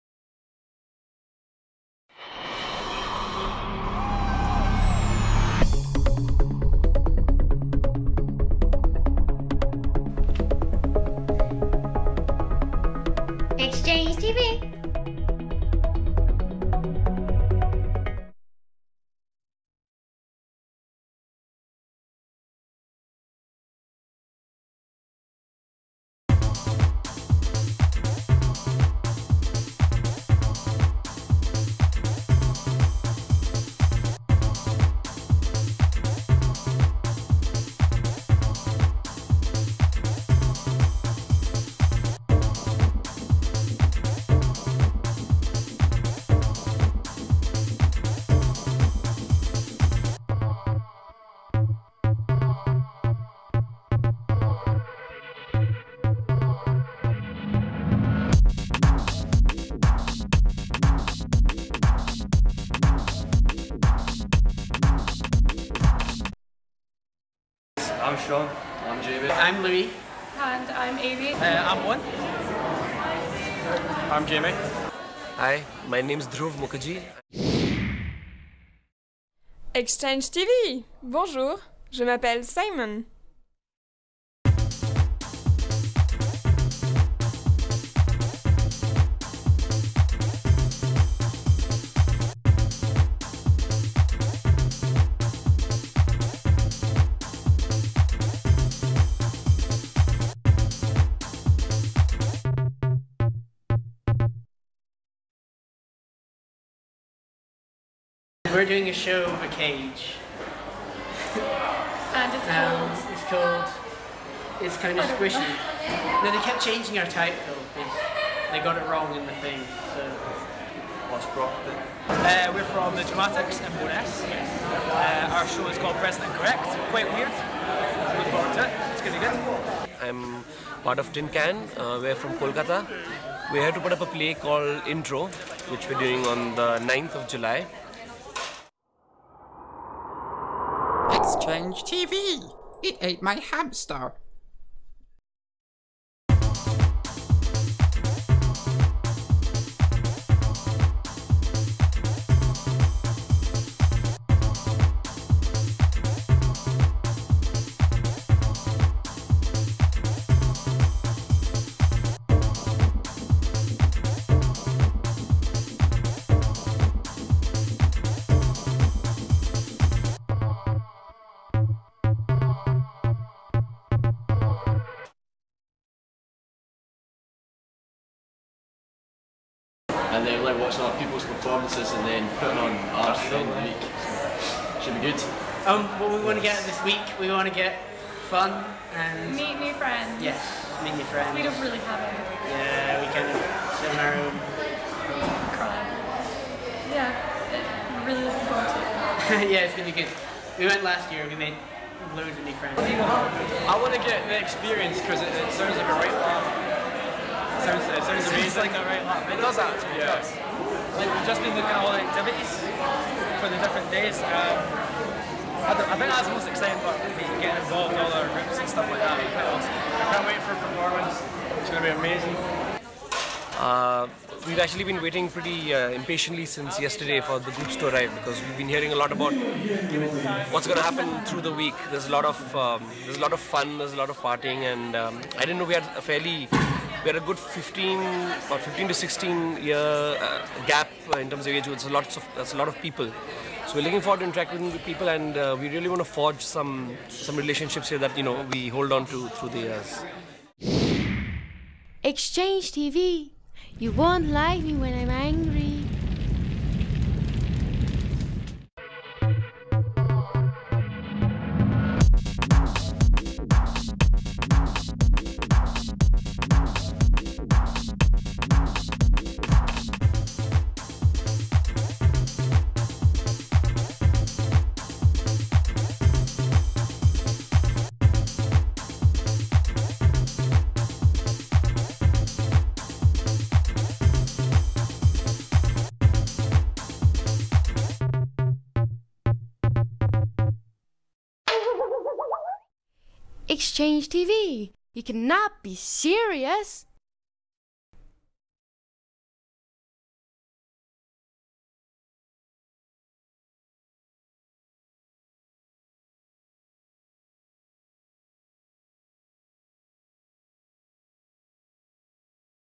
ExchangeTV 2009 - Monday
First day of Exchange09 at Macrobert, Stirling - we meet some of the participants and get a glimpse into what will be happening throughout the week.